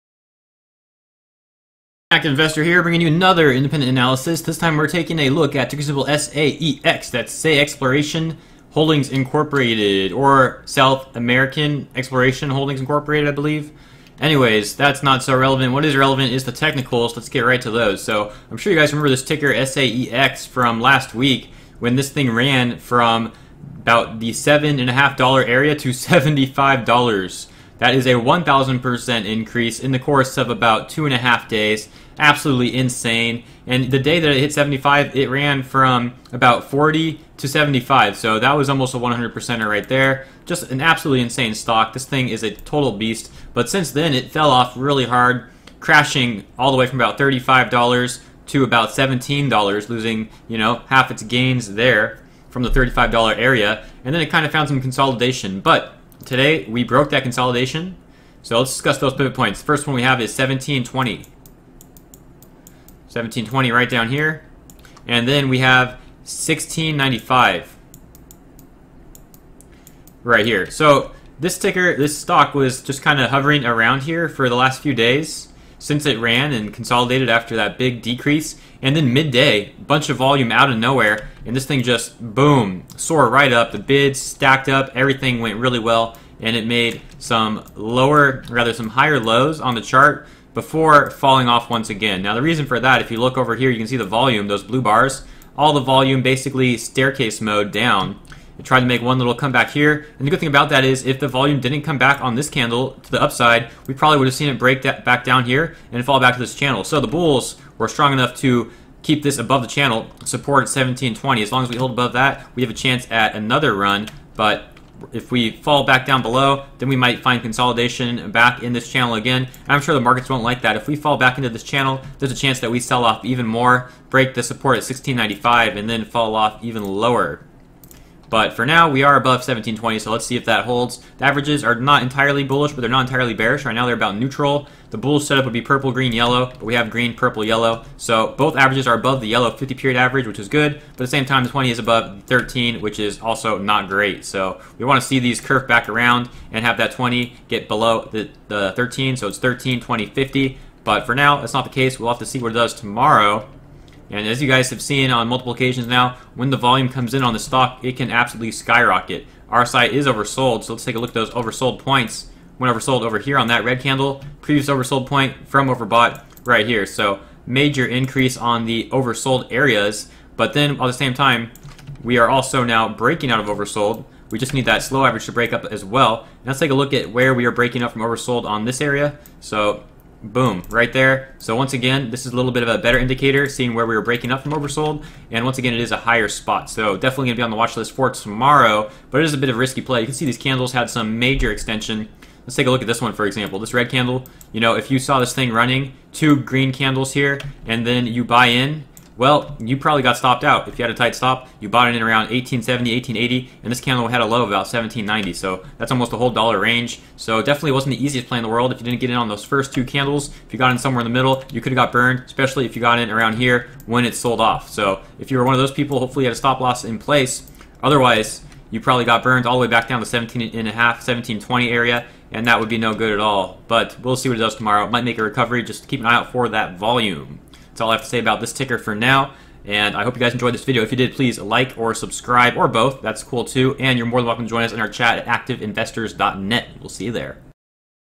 $SAEX - SAExploration - Independent Analysis (2) 8/10/16
Technical Analysis on $SAEX.
My overall sentiments, support and resistance levels, and predictions for upcoming price movements.
We offer a chat room,  live stream, and much more.